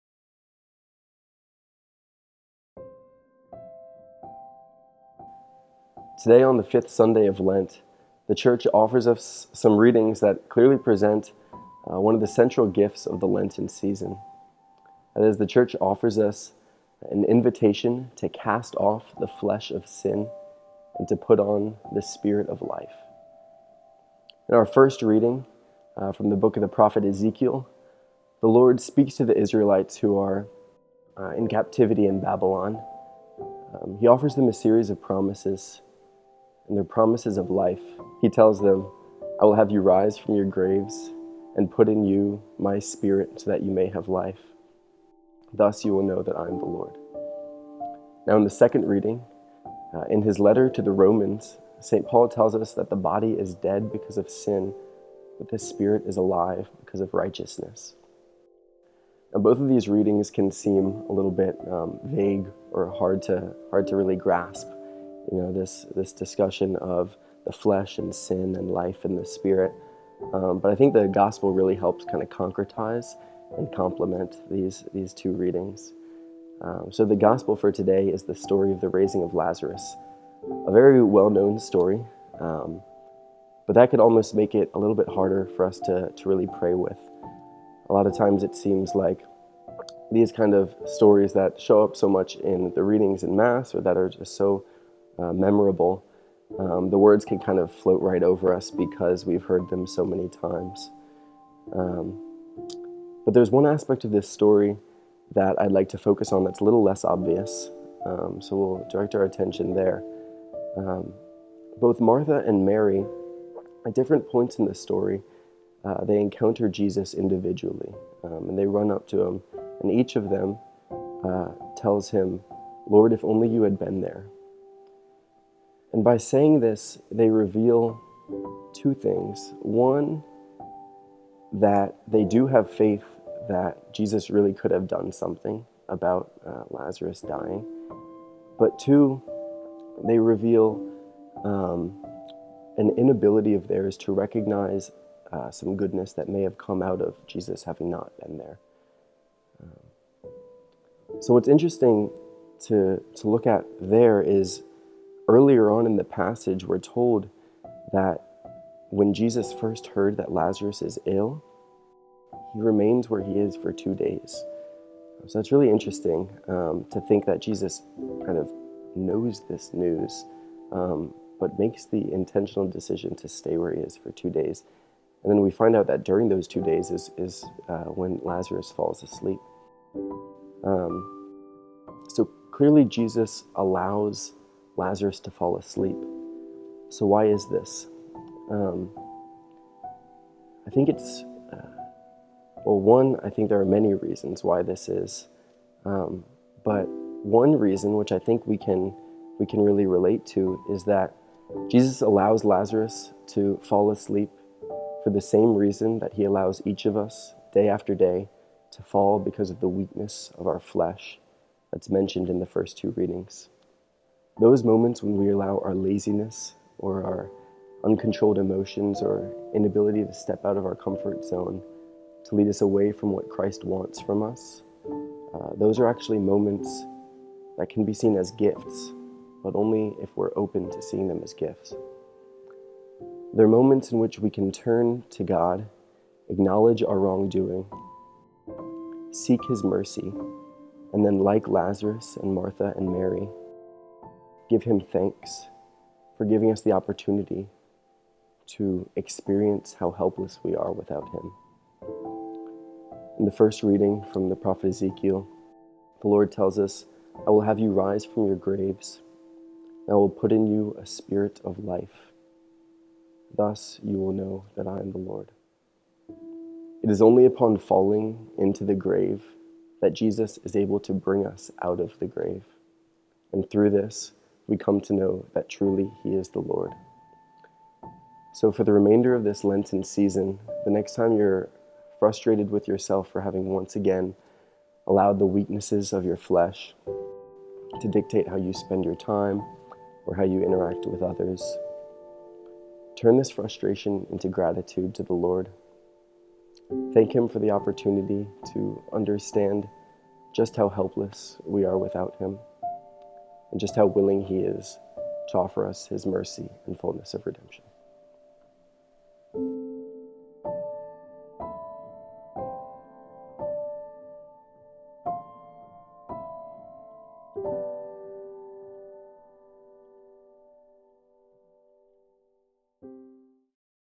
A Lenten Journey 2023 - Reflection for the Fifth Sunday of Lent, 3/26/2023
Accompany the seminarians of Holy Trinity Seminary on A Lenten Journey. Seminarian John Ungarino, a 1st year pre-theologian for the Diocese of Austin, offers his reflection for the Fifth Sunday of Lent.